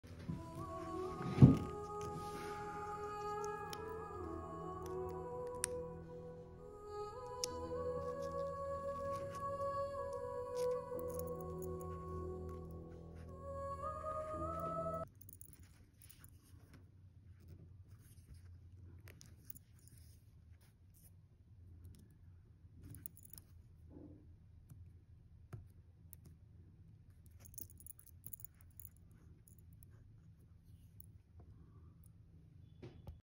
How to insert a photo in a heart locket pendant(last step)
The last step in the photo insertion is to push the photo inside (at first push down and then up), start with a finger, and finish with a toothpick.